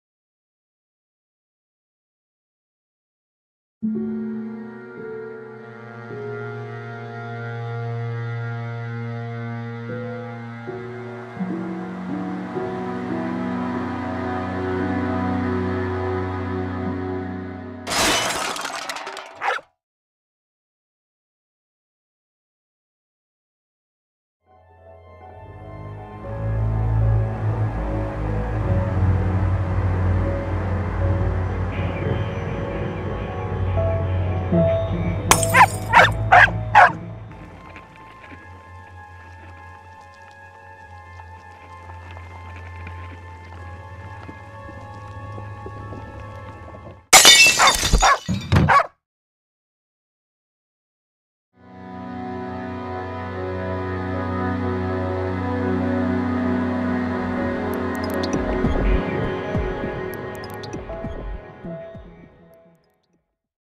Man/Dog
A man vs. a dog. A challenge made to me by a friend to produce a single shot video in an hour and put it on YouTube. It took me longer than an hour (about 2-3), but I got it done, so here it is.